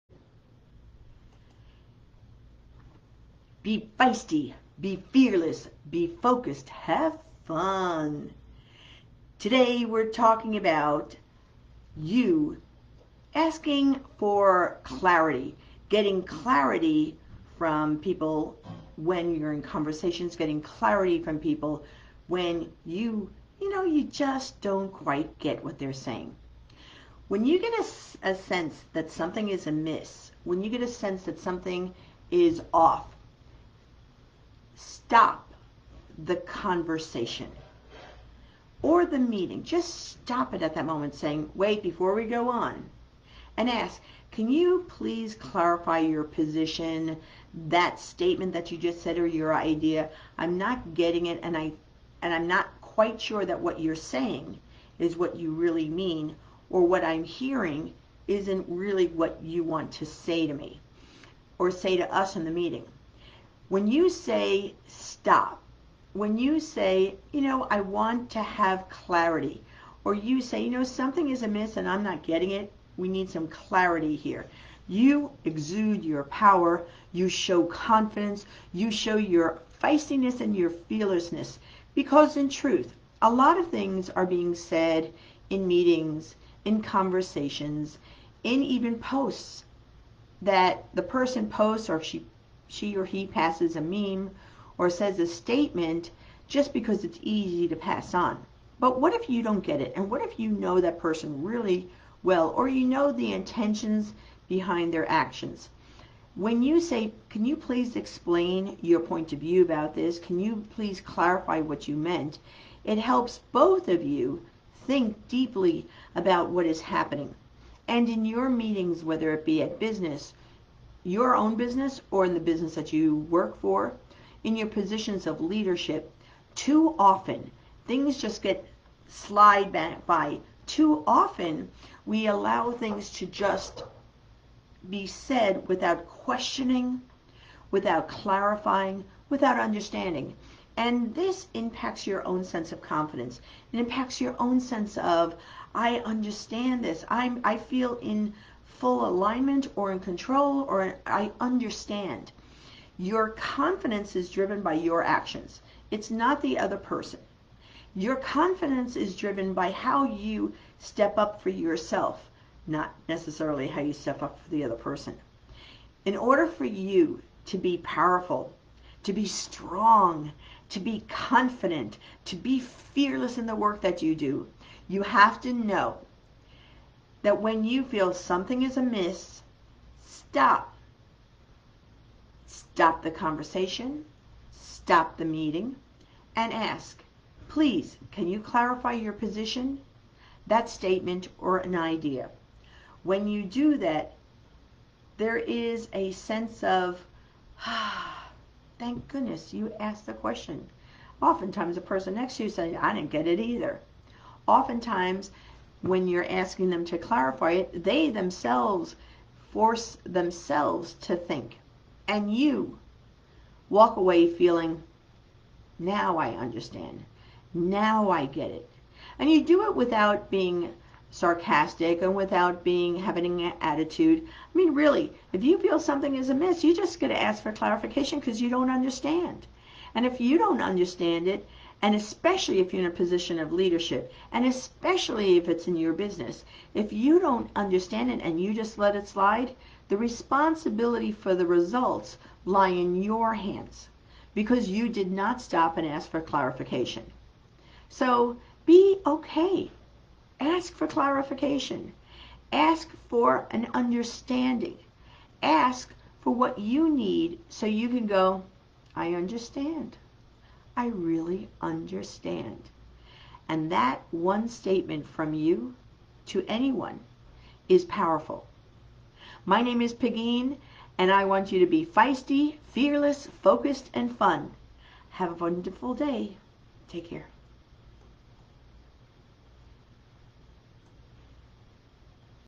Be Feisty Ask for Clarification
Be feisty, be fearless, be focused, and have fun Pegine shares tips for women on leadership, business, and confidence.  It is Friday,  Wear RED to Remember Everyone Deployed.